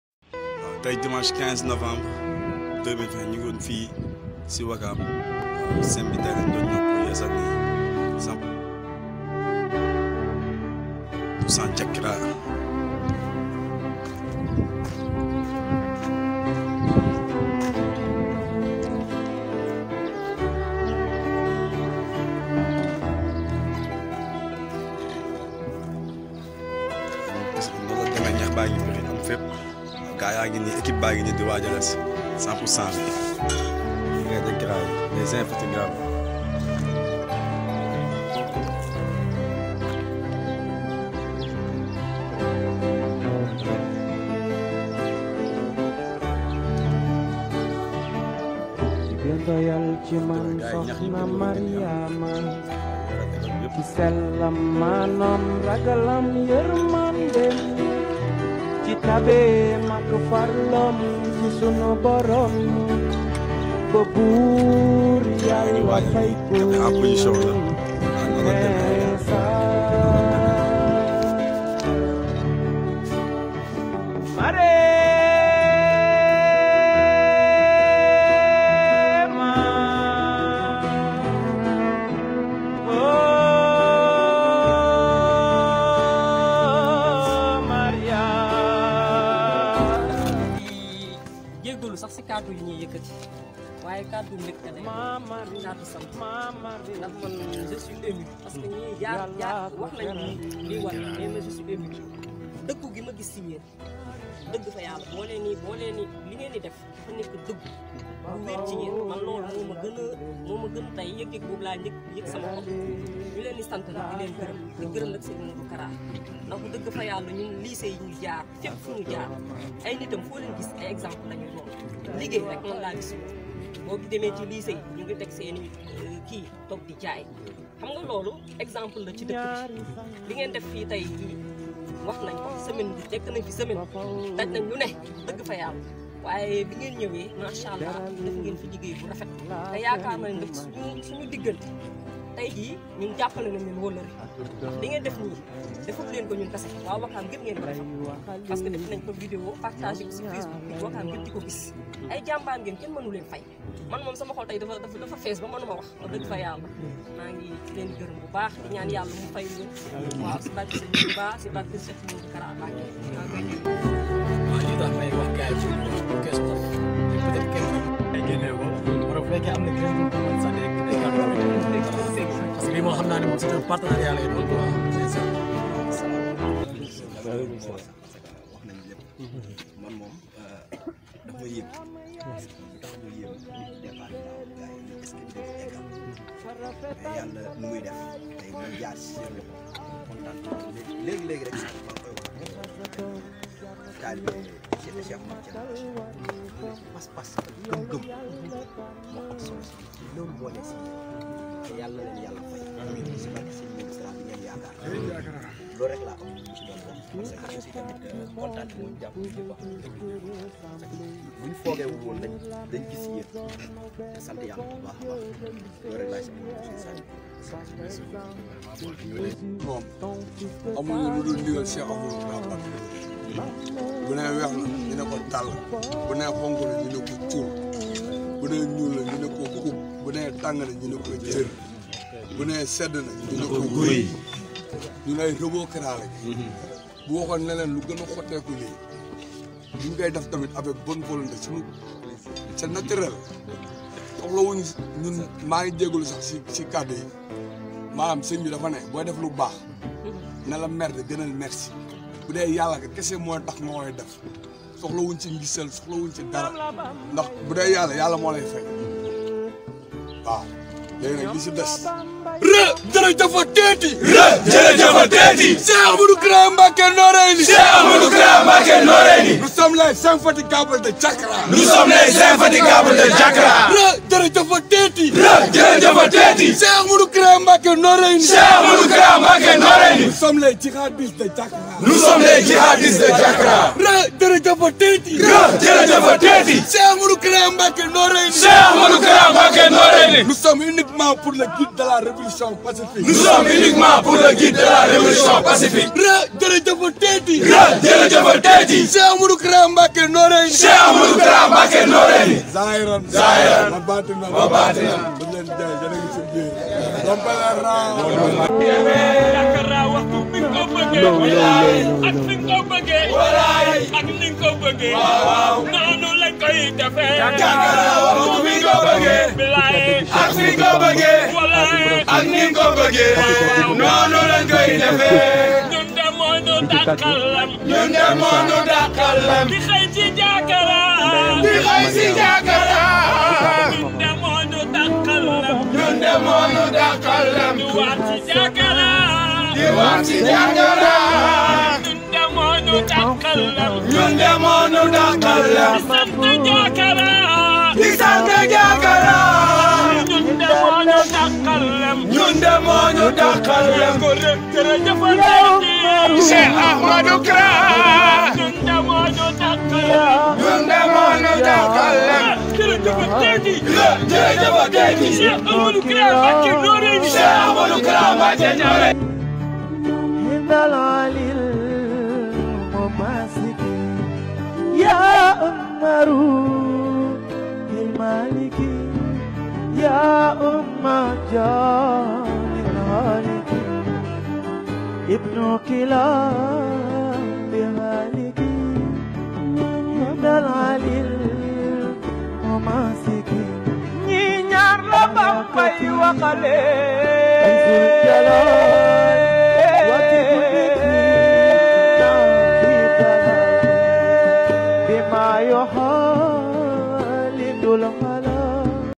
Nettoyage du CEM de Ouakam par les talibés de cheikh Ahmadou Kara le guide de la révolution
Nettoyage du CEM de Ouakam

Oeuvré pour Dieu à travers les hommes
RE DIEUREUDIEUFEUTETI CHEIKH AHMADOU KARA MBACKÉ NOREYNI ZAHIRANE WA BATINANE